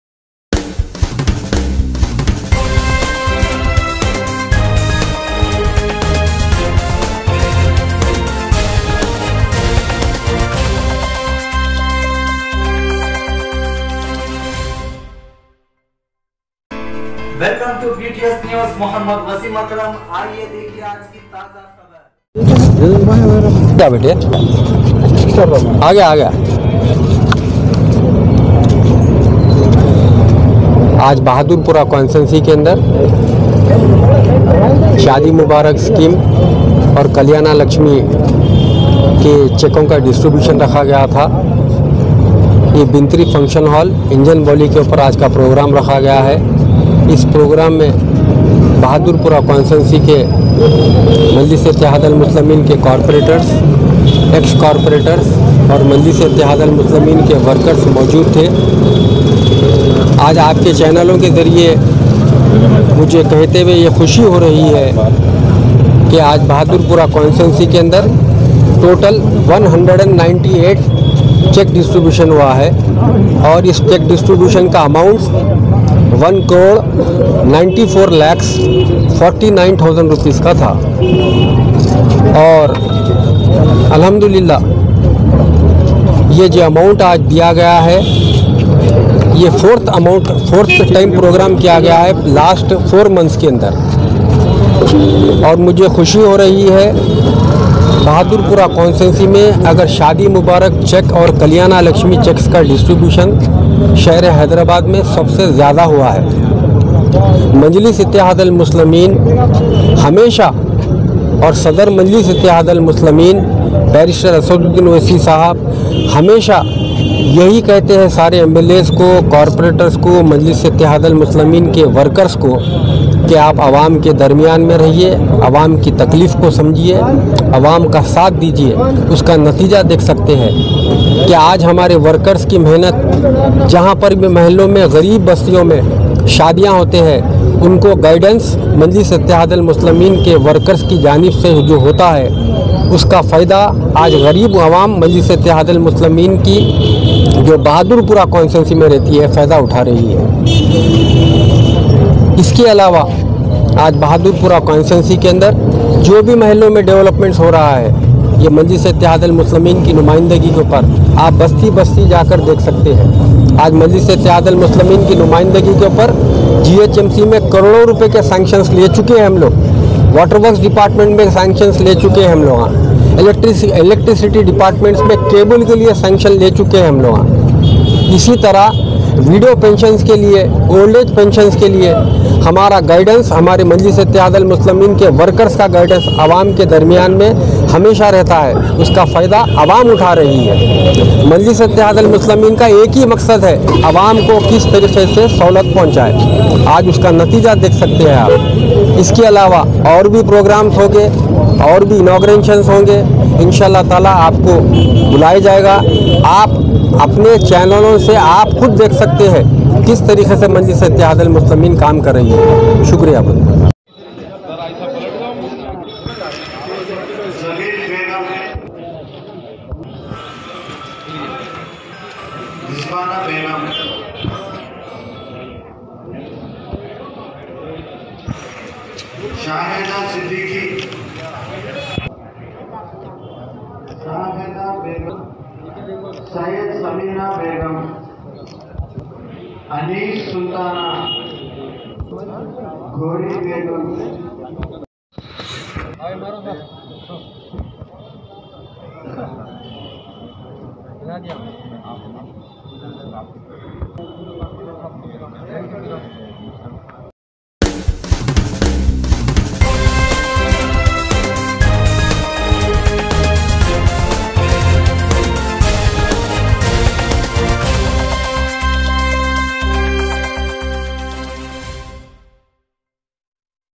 BTS NEWS AIMIM Bahadurpura MLA Nawab Mohd Moazam Khan today along with Bahadurpura MRO officials &
AIMIM Bahadurpura MLA Nawab Mohd Moazam Khan today along with Bahadurpura MRO officials & AIMIM Corporators distributed Shaadi Mubarak & Kalyan Lakshmi cheques among 198 beneficiaries under Bahadurpura Constituency. Total amount of cheques is 1,94,49,736₹.